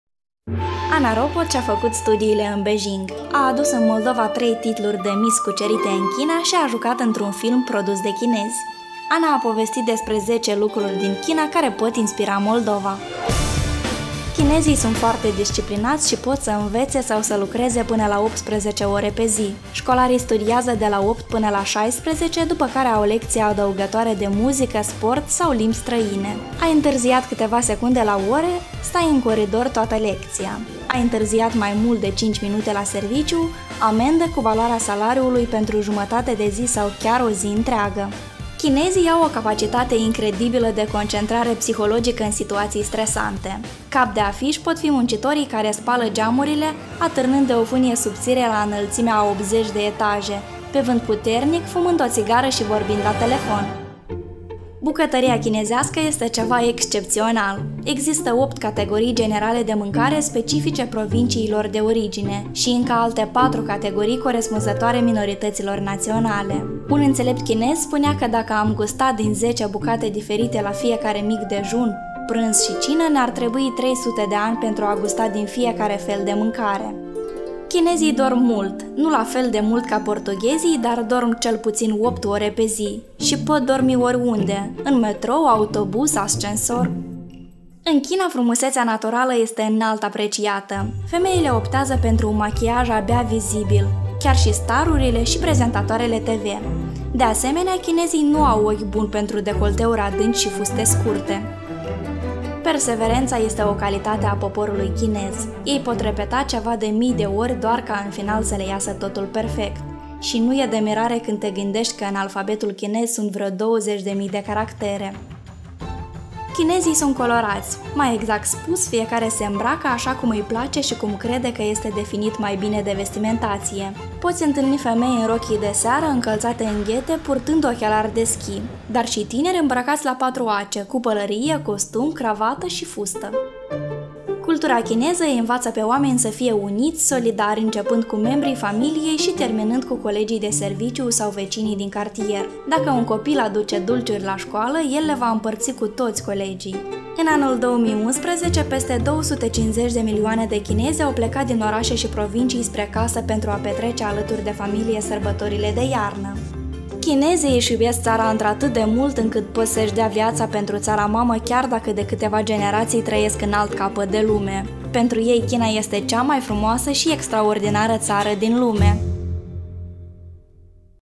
Videografic | 10 lucruri din China care pot inspira Moldova: Ana Ropot
Ana Ropot este o tânără din Moldova care şi-a făcut studiile la Beijing, a adus în Moldova trei titluri de Miss cucerite în China și a jucat într-un film produs de chinezi, „Legenda Drumului Mătăsii”. Ea a scris pentru Știripozitive.eu 10 lucruri pe care trebuie să le învățăm de la chinezi.